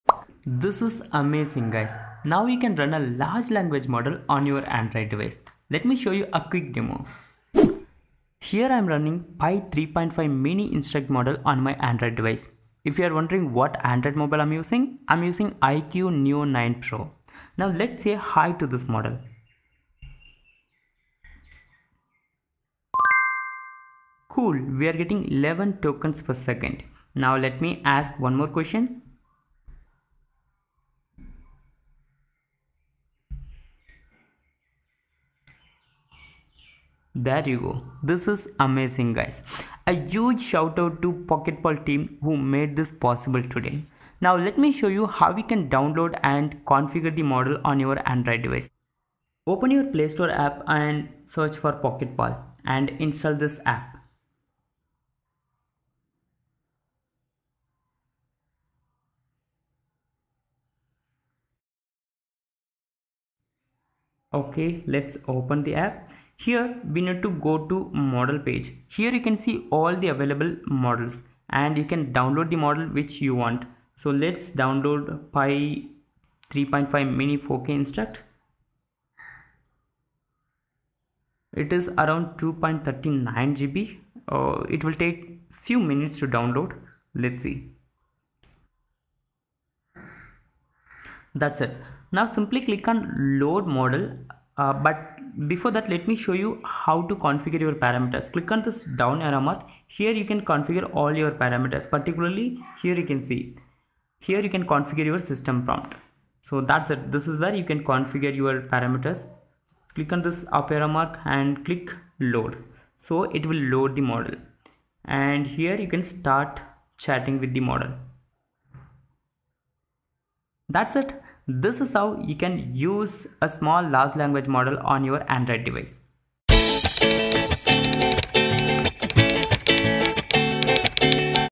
Your Offline AI Assistant on Android! : PocketPal AI
Here’s a beautiful and detailed description for your PocketPal AI video:

Welcome to PocketPal AI – your personal, pocket-sized AI assistant that works without an internet connection!

In this video, I walk you through the simple steps to download and configure PocketPal AI on your Android device. Whether you’re on the go or in a low-connectivity area, PocketPal AI lets you interact with advanced small language models (SLMs) directly from your phone. No Wi-Fi? No problem! Experience the power of AI without the need for any data or internet connection.

What you’ll see in this video:

How to download PocketPal AI from the Play Store
Step-by-step guide to configuring the AI models
A live demo of PocketPal AI in action
PocketPal AI is designed for both iOS and Android, giving you the ability to run AI models right from your device, wherever you are. Whether you need quick responses, help with tasks, or simply want to experiment with AI on your phone, PocketPal AI has got you covered.

If you're excited to explore AI technology that works offline, make sure to watch the full demo and see just how easy it is to set up your personal AI assistant today!

Download PocketPal AI and carry the future in your pocket!